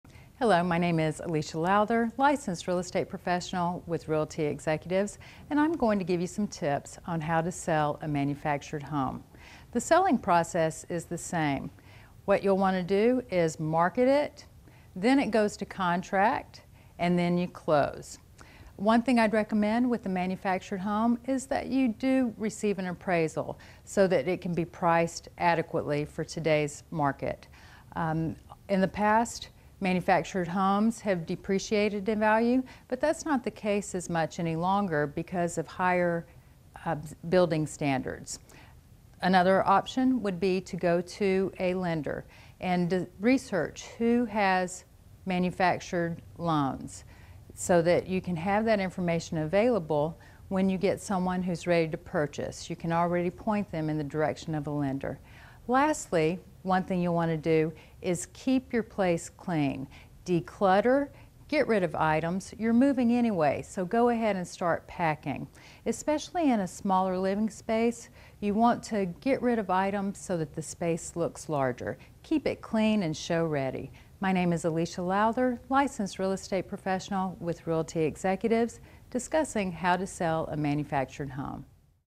Buying & Selling Homes : How to Sell a Manufactured Home
Selling a manufactured home is similar to selling any other home, but an appraisal is crucial to get an accurate fair market price. Keep up with the current market when selling a manufactured home with information from an experienced real estate agent in this free video on selling homes.

Expert: Alicia Lowther
Bio: Alicia Lowther prides herself on providing excellent service for both home buyers and sellers in Pinellas County.